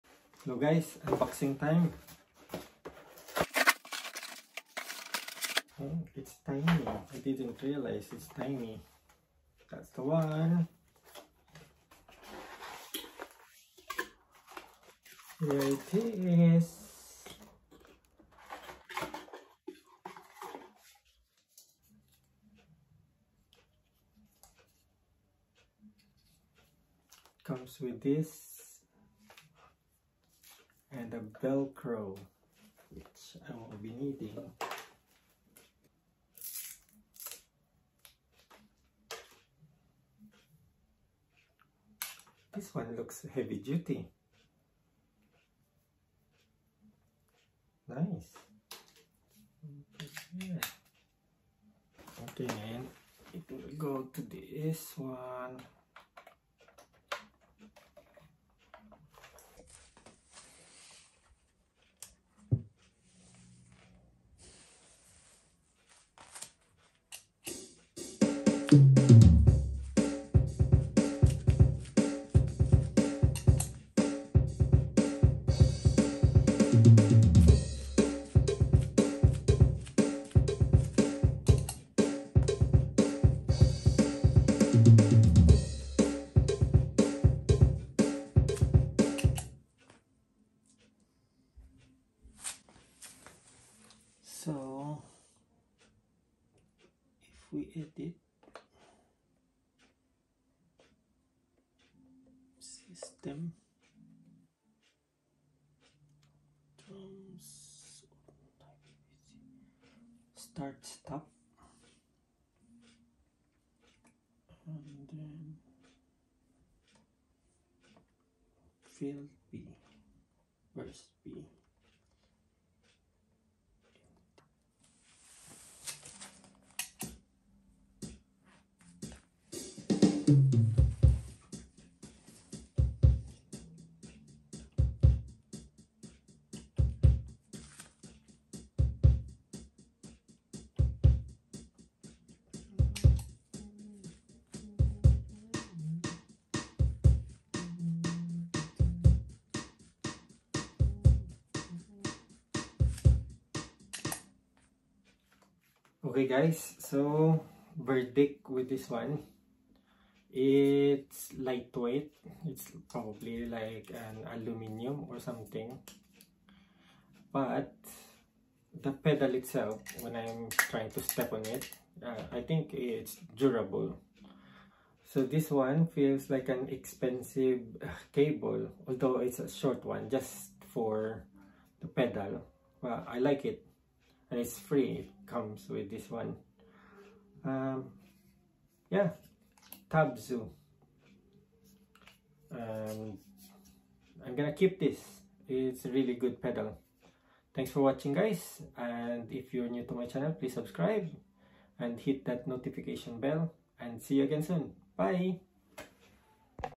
Tavsou Dual Footswitch Pedal Unboxing and Demo alternative footswitch for Boss Dr Rhythm DR3
Tavsou Dual Footswitch Pedal For Guitar Effect Pedal With TRS Cable Non-latching bought from Amazon an alternative footswitch for your Boss Dr Rhythm DR3